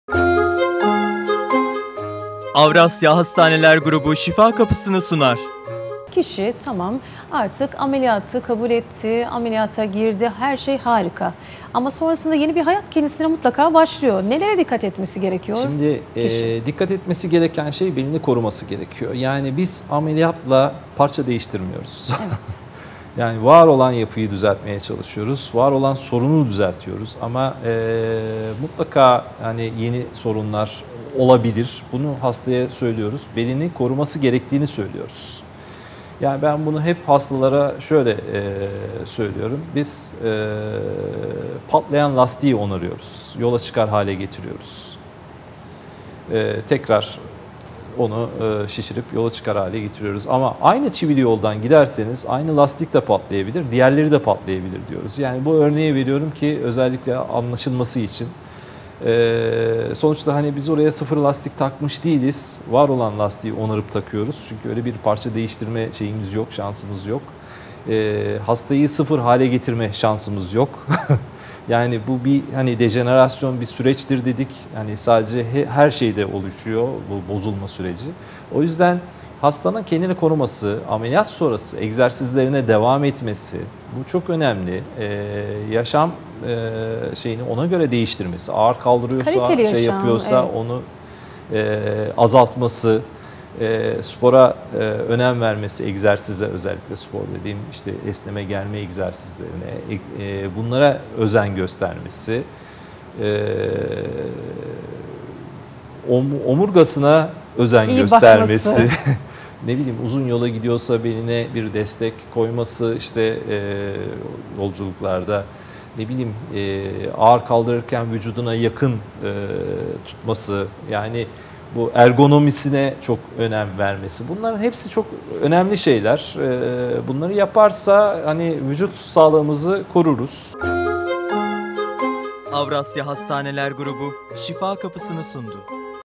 Bel Fıtığı Ameliyatı Sonrası Nelere Dikkat Edilmesi Gerekir?
Avrasya Hastanesi Zeytinburnu Beyin ve Sinir Cerrahisi Uzmanı Doç. Dr. Çetin Ayhan Evliyaoğlu, bel fıtığı ameliyatı sonrası dikkat edilmesi gerekenler hakkında bilgilendiriyor.